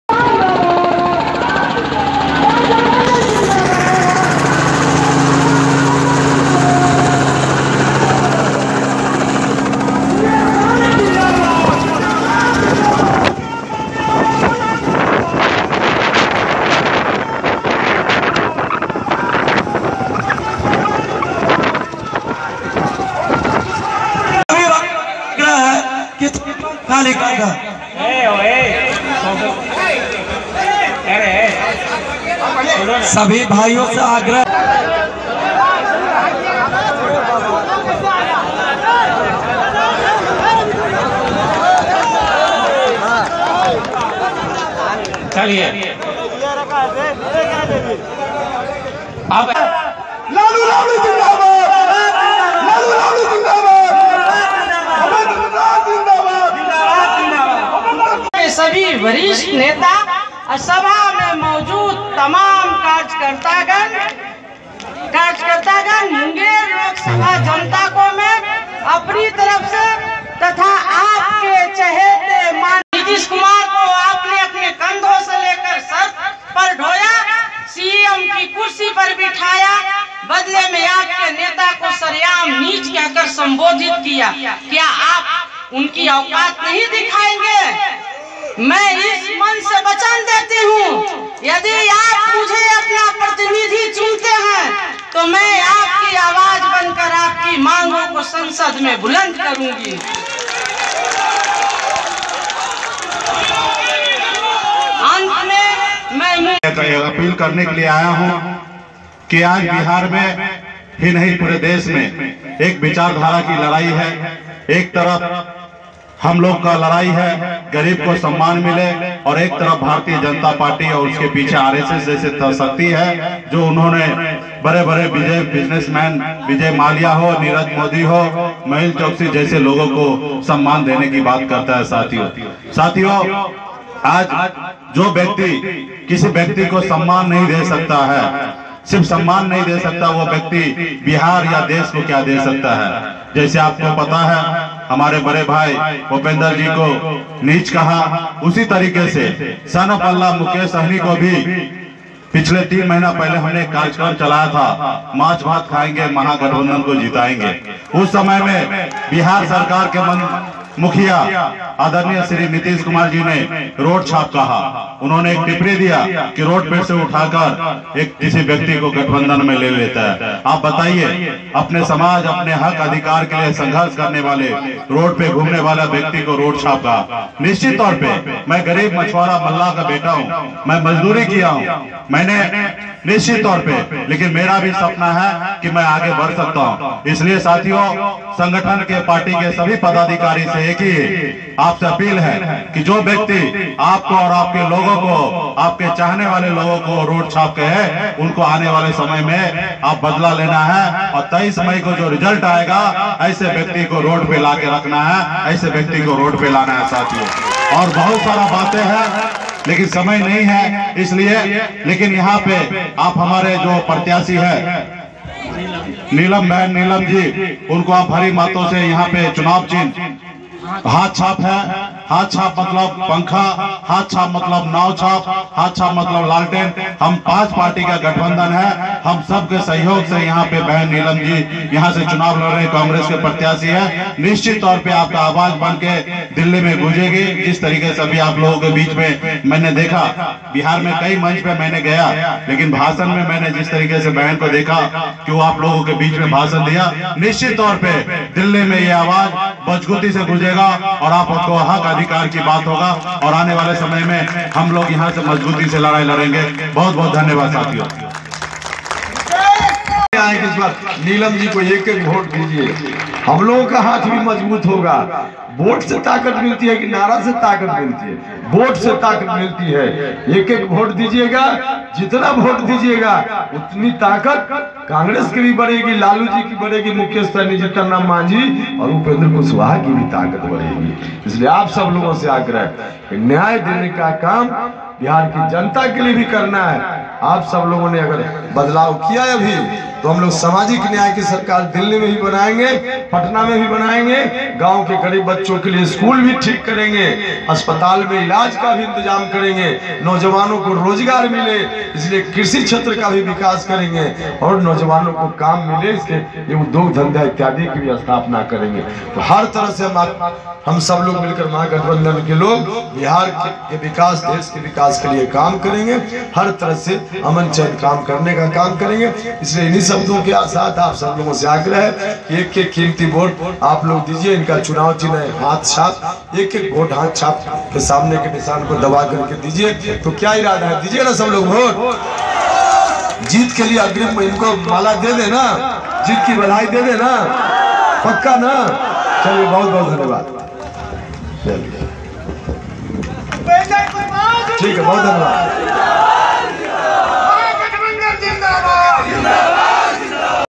उड़नखटोला से पहुंचे उपेंद्र कुशवाहा और सन ऑफ मल्लाह,नीलम देवी के पक्ष में की जबरदस्त चुनावी सभा
मुंगेर लोकसभा के महागठबंधन की उम्मीदवार za नीलम देवी के समर्थन में जनसभा को संबोधित करने रालोसपा के राष्ट्रीय अध्यक्ष उपेंद्र कुशवाहा और वीआईपी के राष्ट्रीय अध्यक्ष मुकेश सहनी लखीसराय के हलसी पहुंचे। दोनों नेताओं ने एनडीए पर जमकर निशाना साधा और मुंगेर से महागठबंधन की उम्मिदवार को भारी मतों से विजयी बनाने का आह्वान आम लोगों से किया।